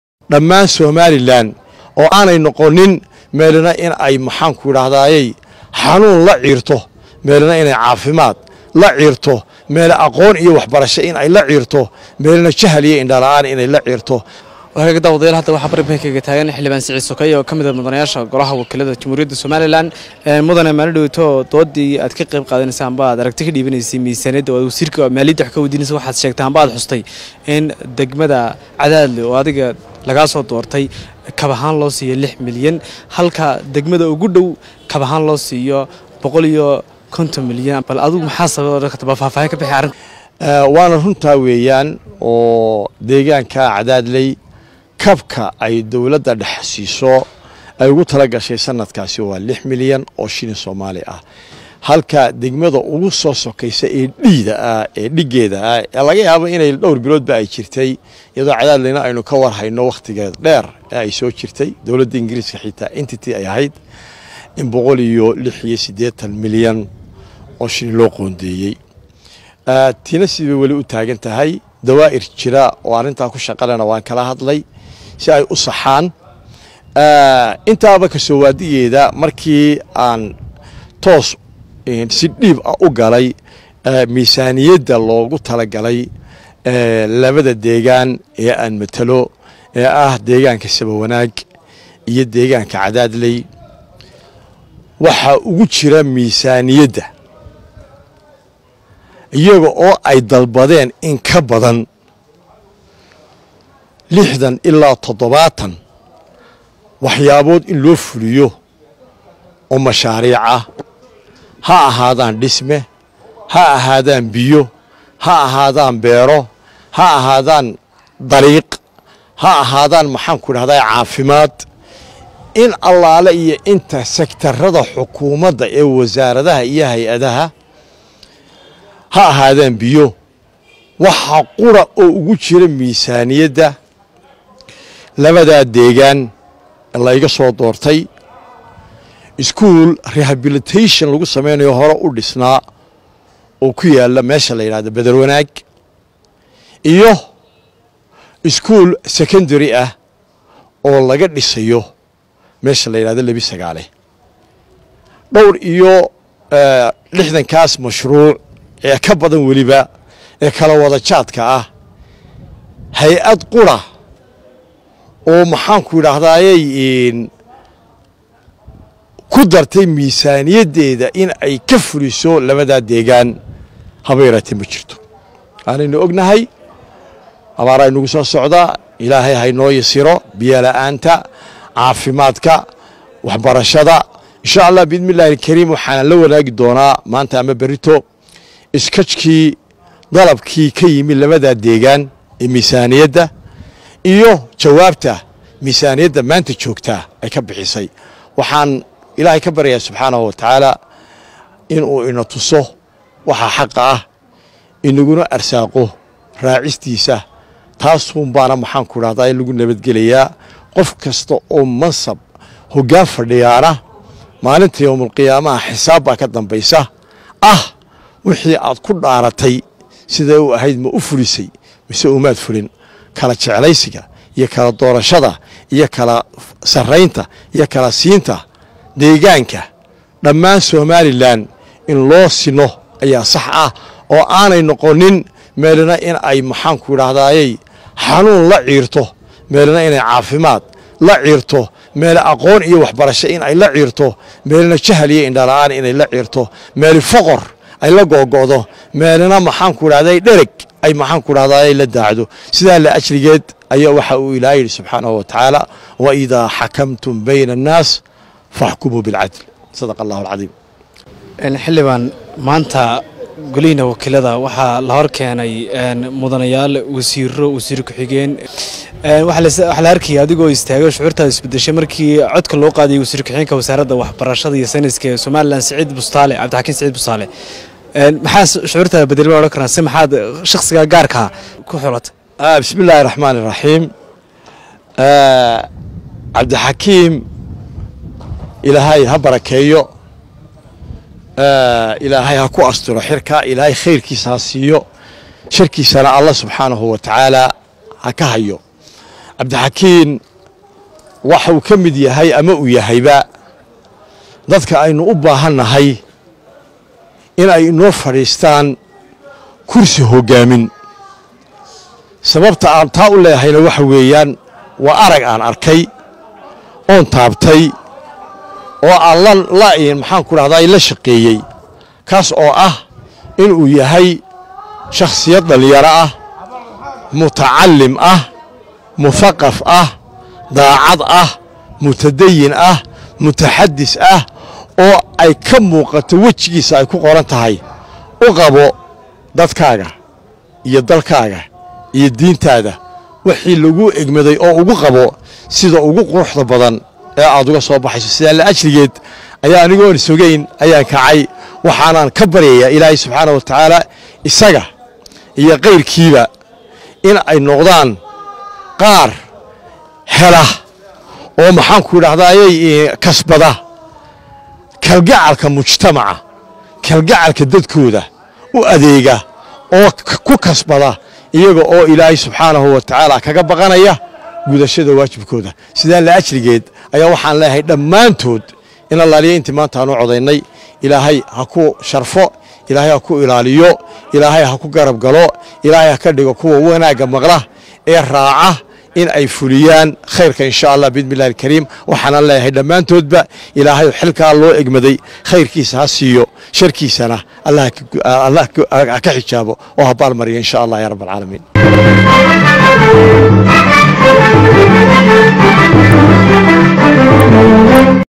Sadbursiga Miisaaniyadda Ee Degmooyinka Dalka Qaarkood Halka......."Xildhibaan Siciid Sokeeye"....
Cadaalad darro weyn baa ka muuqata Miisaaniyadda qaranka 2022...